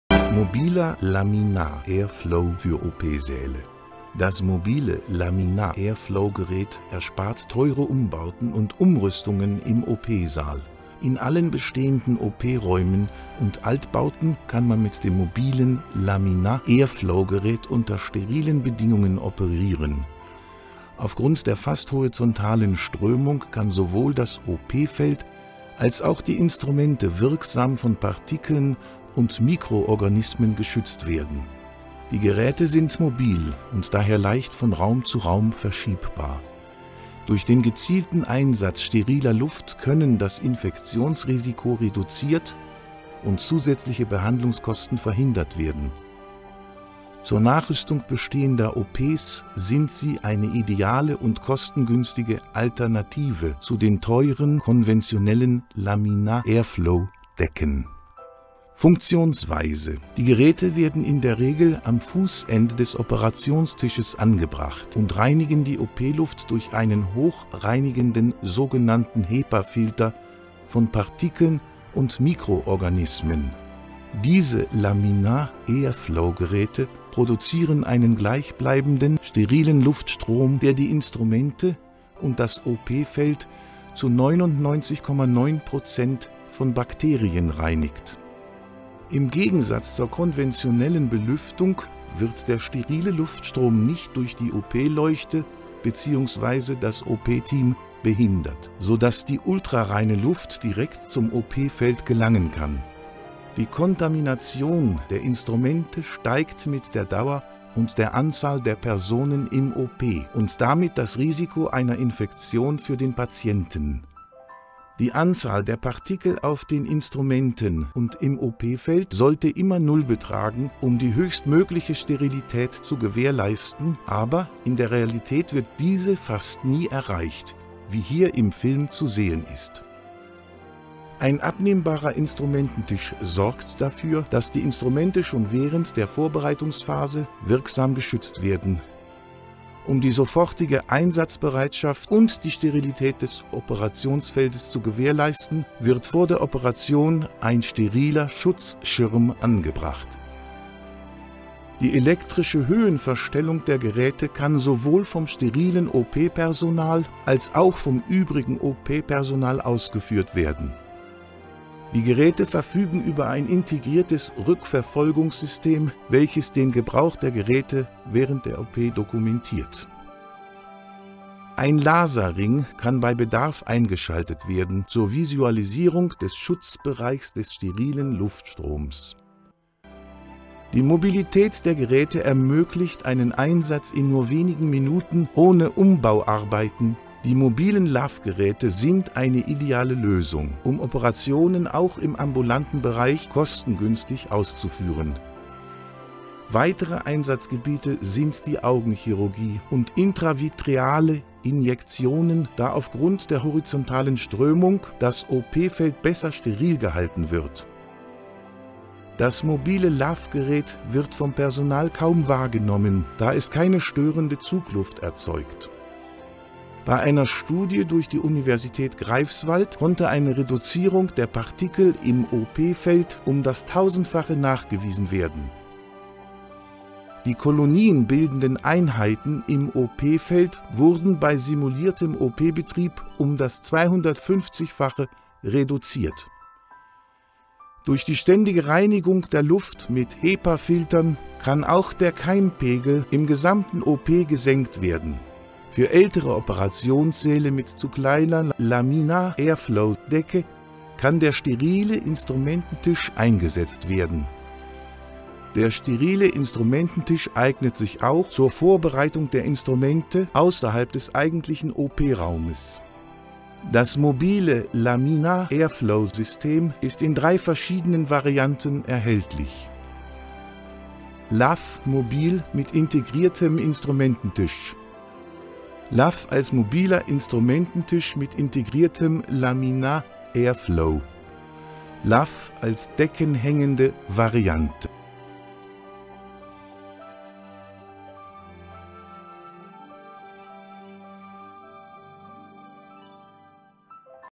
Raumluft OP mit mobilem TAV Geraet
Für OP Säle oder Eingriffsräume die bisher noch nicht mit einer entsprechenden keimarmen Zuluftdecke ausgerüstet sind gibt es ein mobiles Laminar Air Flow Gerät welche unabhängig von der Art der bisherigen Klimatisierung genutzt werden kann.Die mobilen Laminar Air Flow Systeme sind ideal um bestehende OP- und Eingriffsräume kostengünstig und sicher aufzurüsten.